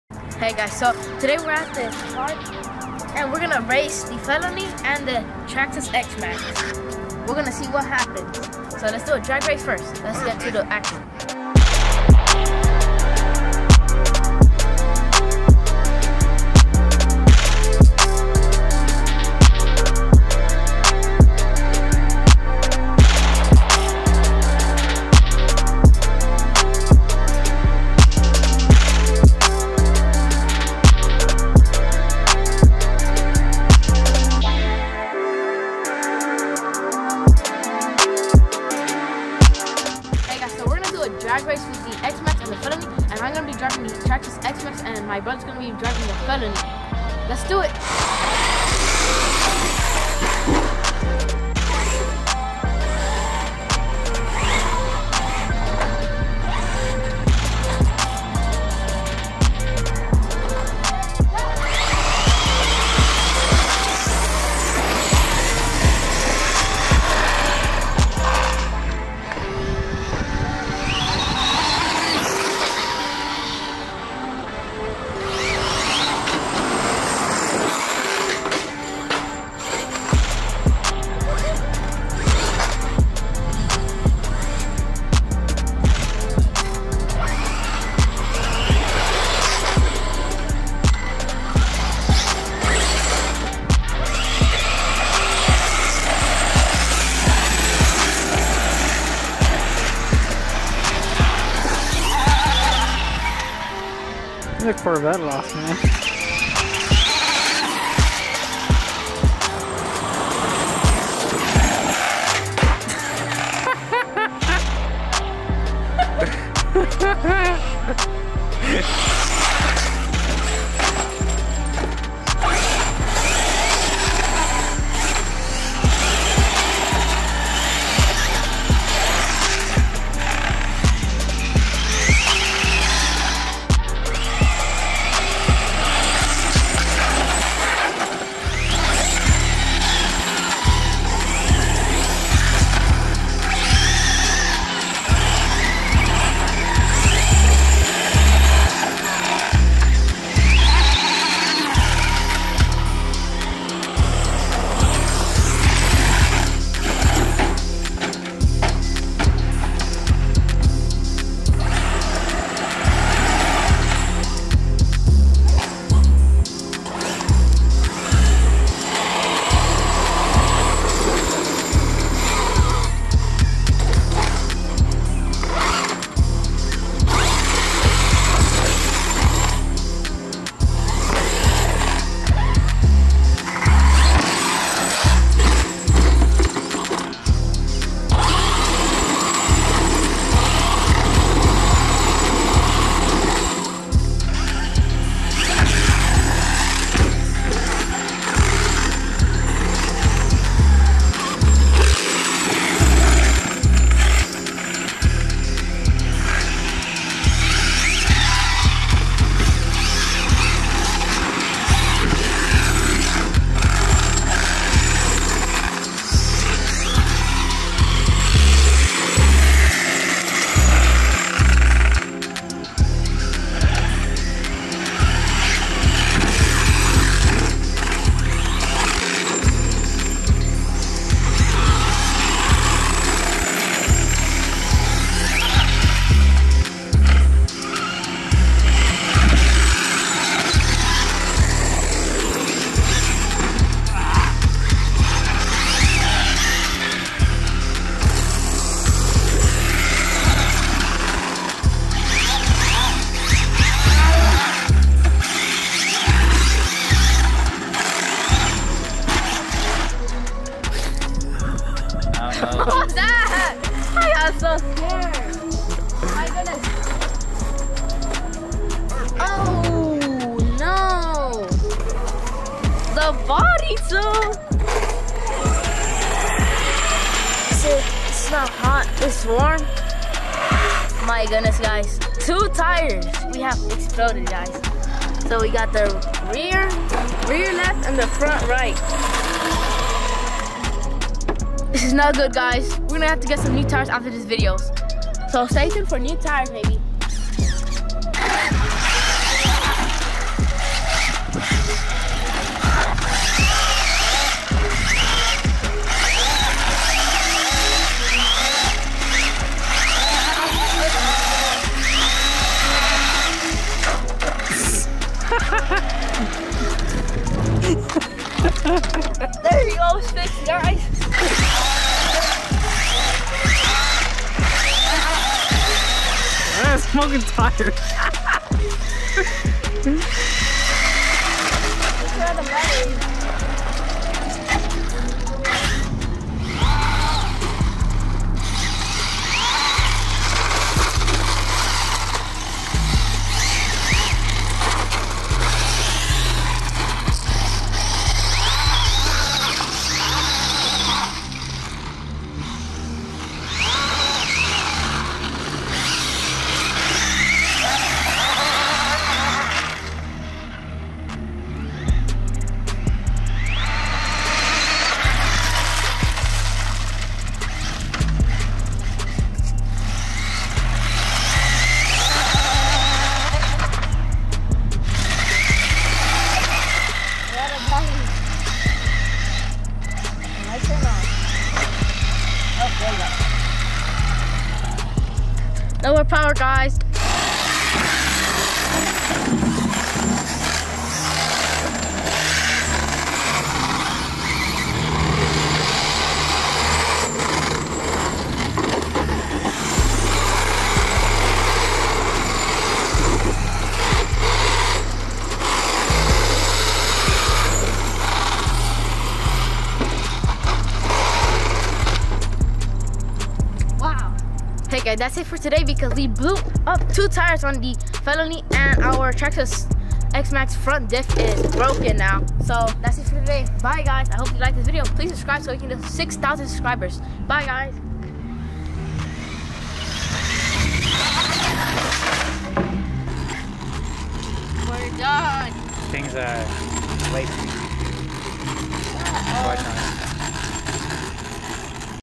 TOTAL DESTRUCTION XMAXX VS ARRMA FELONY 8S CRASHES OUT CONTROL TRAXXAS XMAXX EXTREME VIDEOS
TRAXXAS VS  ARRMA FELONY 8S TOTAL  DESTRUCTION  EXTREME COMPETETION.